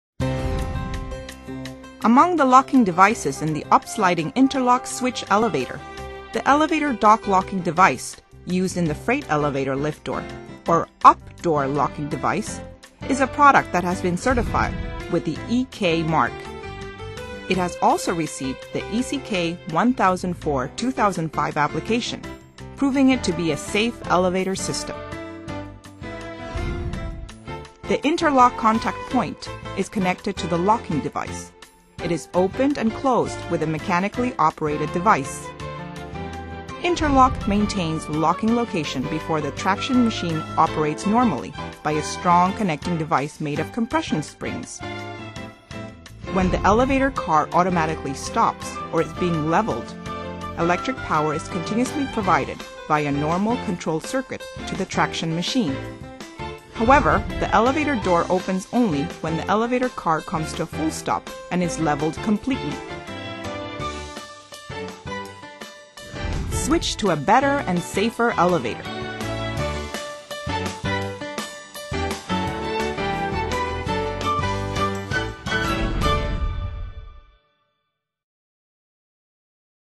Up-sliding interlock switch for use in elevator doors by Dongin electric
Elevator Link Switch is attached to top and bottom of elevator and reduces the speed or cuts the power for preventing elevator to go beyond limited operation distance and also can be used for stage setting or general machine and etc.